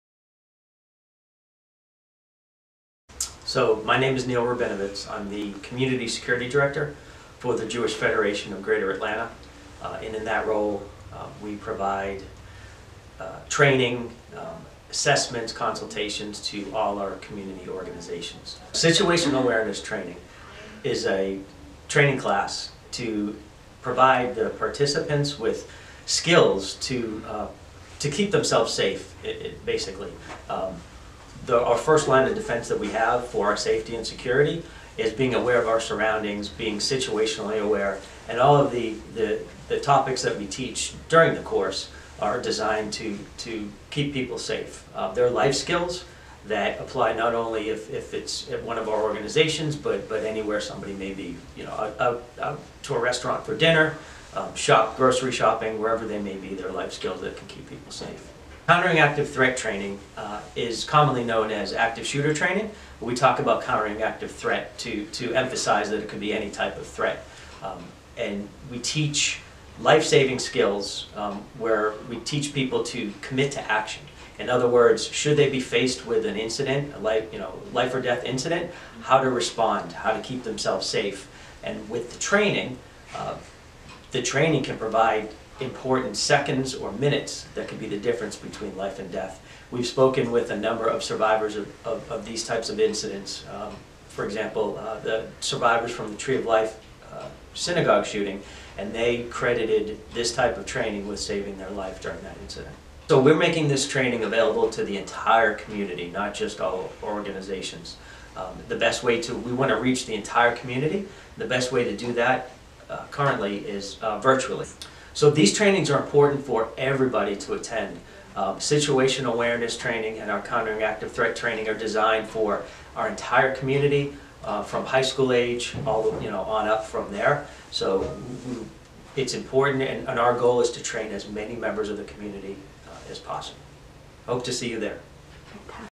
Security Seminars Interview with Neil Rabinovitz
Interested in taking a Security Seminar? Learn more about what it is we do and the man behind the training, Neil Rabinovitz. And if you want to learn more, visit us